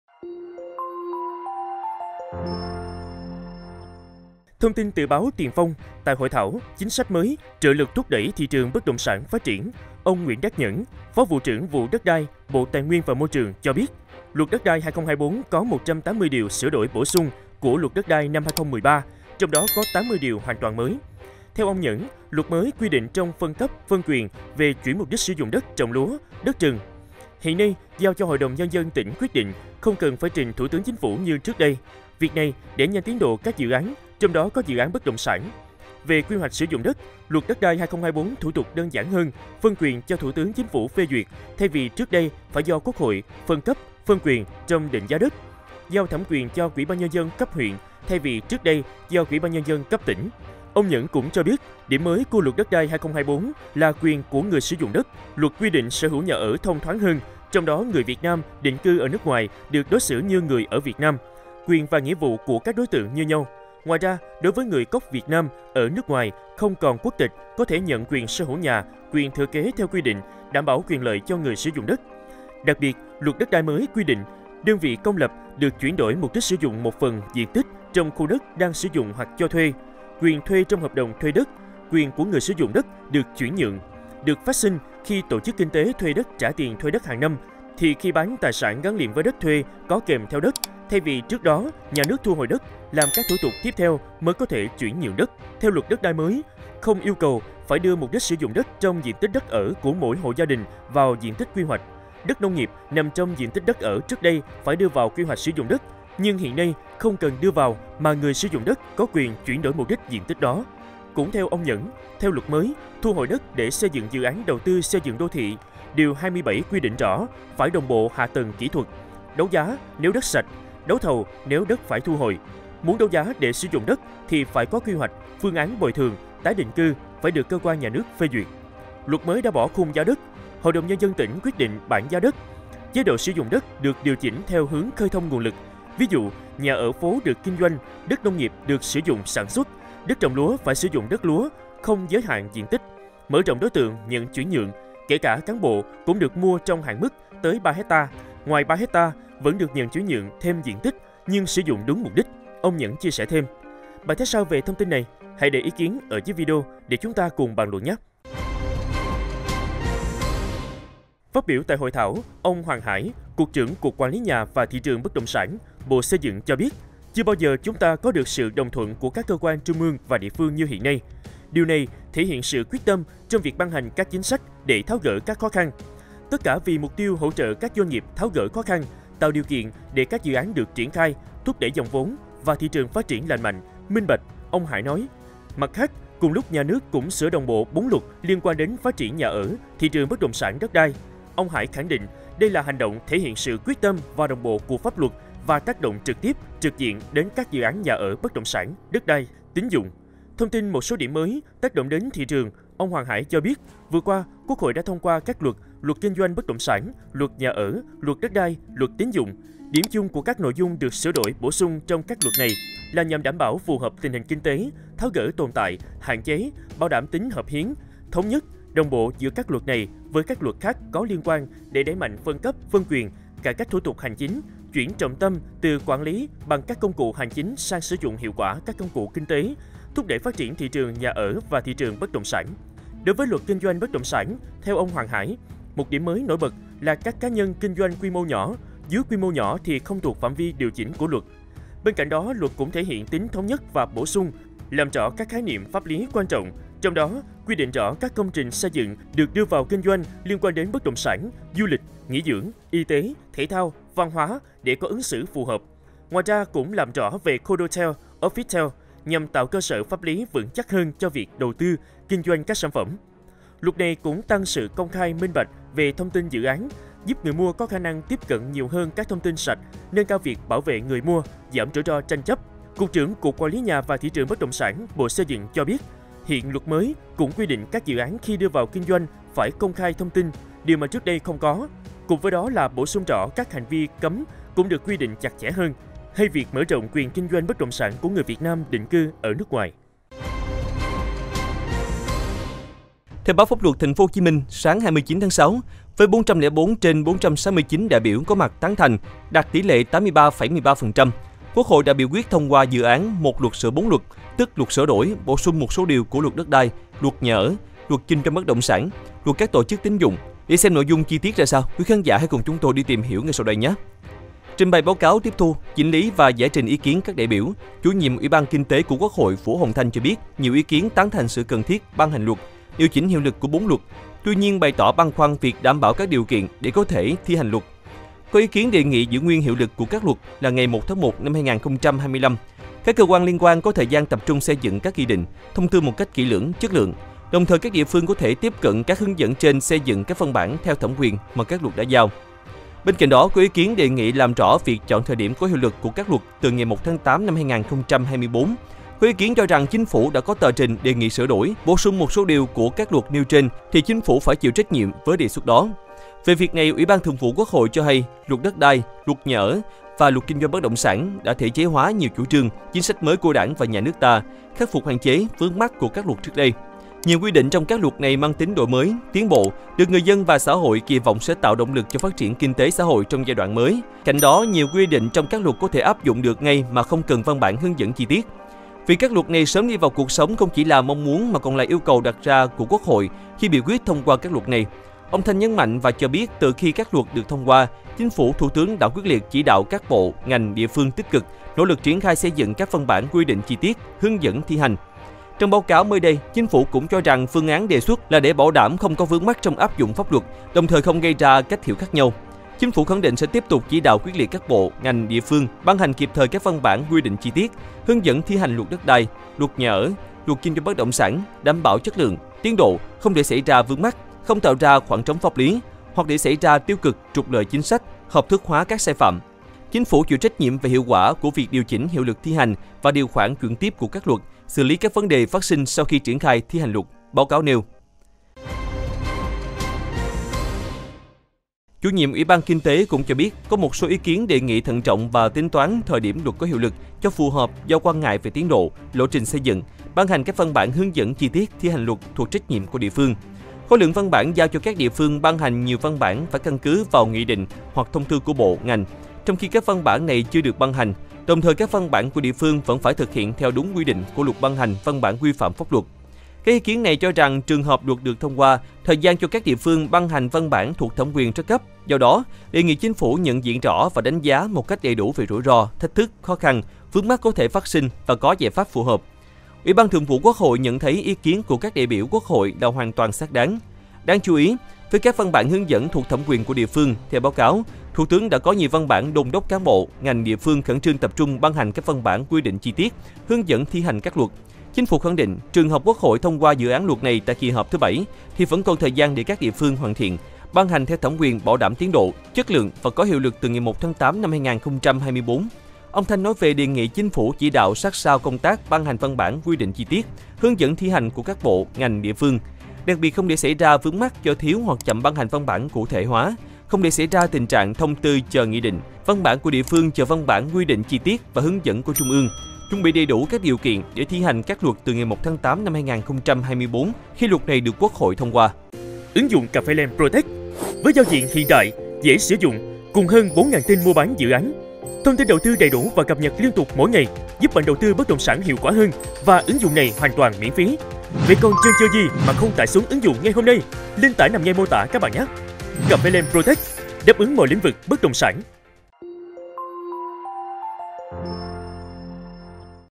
Luật Đất đai sửa đổi có đến 80 ĐIỀU MỚI | CafeLand
Thông tin từ báo Tiền Phong, Tại hội thảo "Chính sách mới - Trợ lực thúc đẩy thị trường bất động sản phát triển", Ông Nguyễn Đắc Nhẫn, Phó Vụ trưởng Vụ Đất đai, Bộ Tài nguyên và Môi trường cho biết, Luật Đất đai 2024 có 180 Điều sửa đổi bổ sung của Luật đất đai năm 2013, trong đó có 80 Điều hoàn toàn mới.